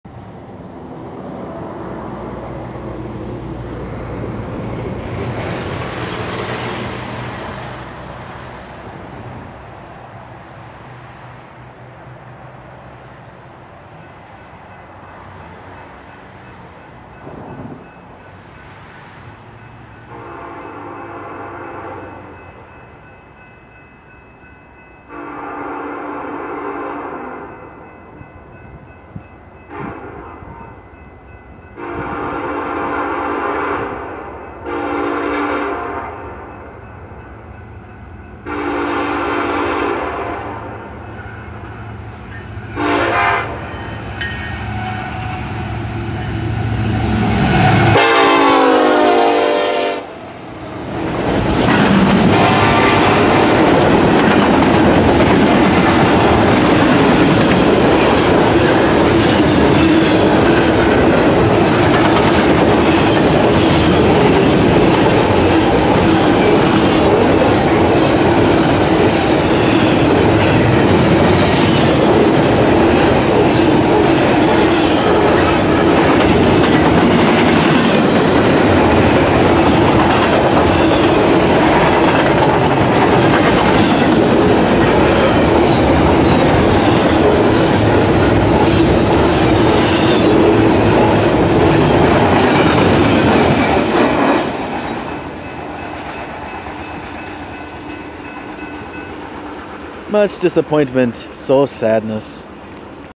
BNSF 8468 East in Mazon, IL 11/23/19
6932... trailing...  AGAIN!!!

EB Stack, Route 47, Mazon, IL, BNSF Chillicothe Sub
BNSF 8468 = SD70ACe - H3 - Raised Letter K5LLA